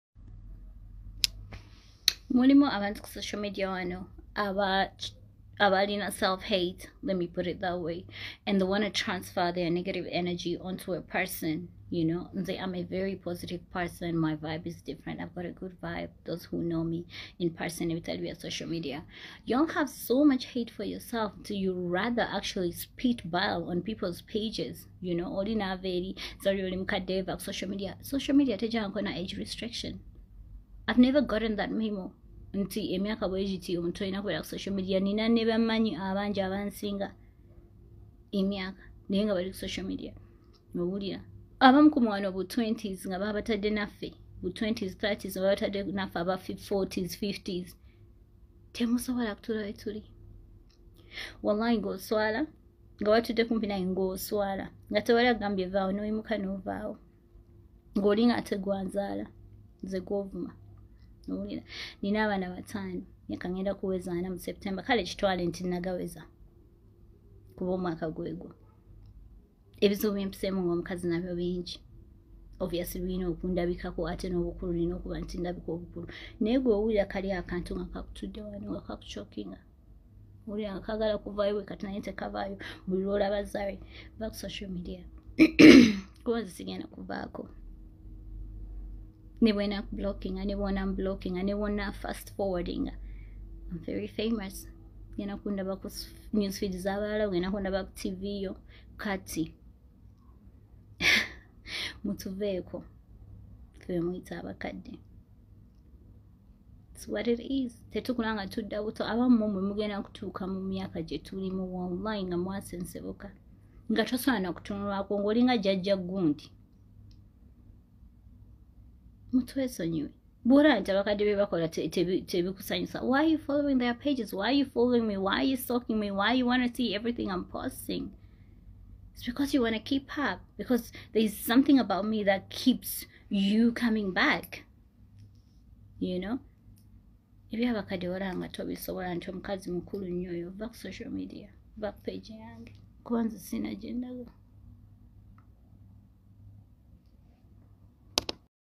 Zari hassan avumye ba fan abamuvuma obukadde
I will not leave social media !!!zari roars